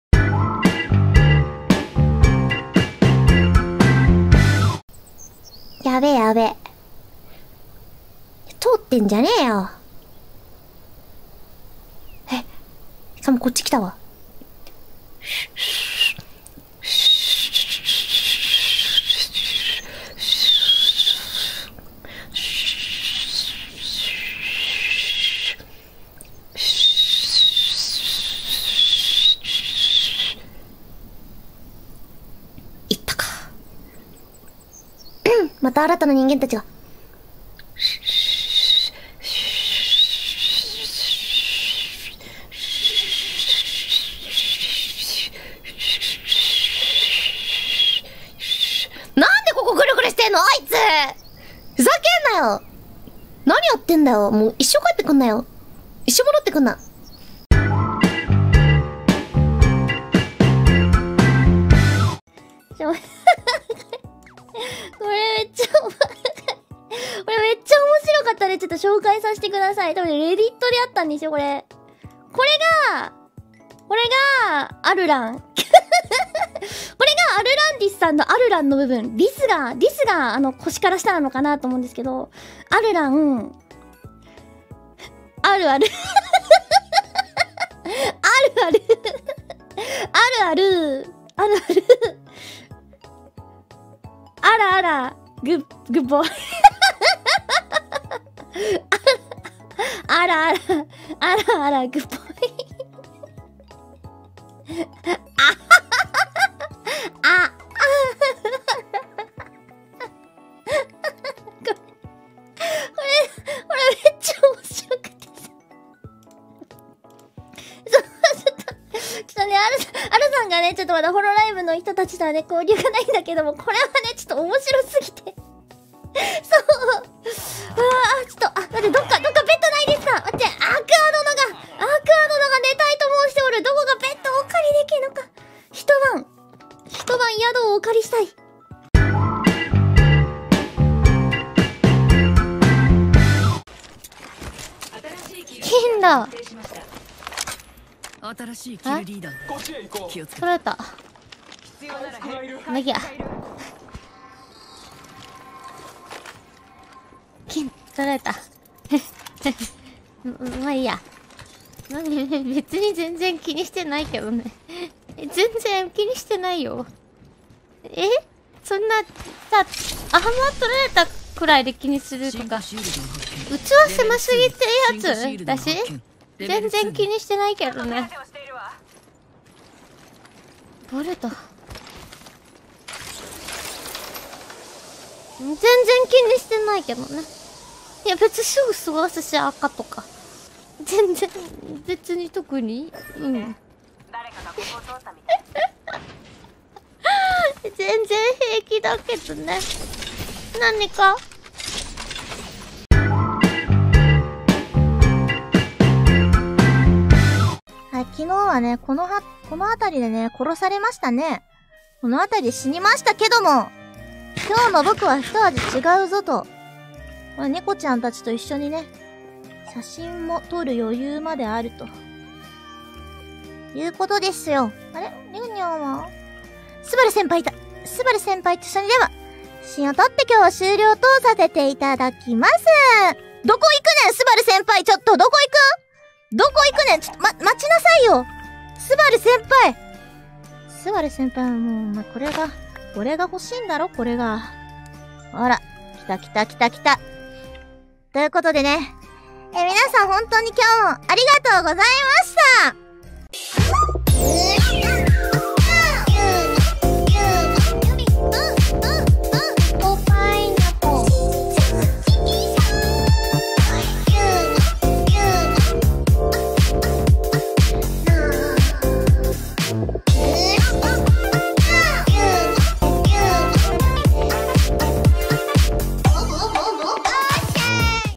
Suisei Whistling Intensifies, Fubuki Aruran Meme, Aqua Doesn't Mind, Kanata and Subaru-senpai
Time Stamp :
00:00 ~ Hoshimachi Suisei
00:57 ~ Shirakami Fubuki
02:18 ~ Minato Aqua
03:23 ~ Amane Kanata